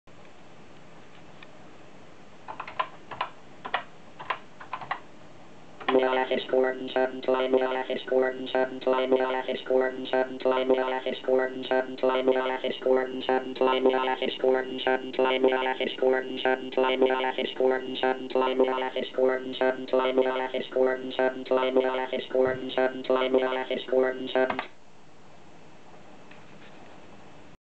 Acorn Atom - Speech Test#1 SP0256-AL2 (Looping sound)
Bringing back to life an old speech pcb...

Speech chip - SP0256-AL2

(yes that;s me in the TV reflection...)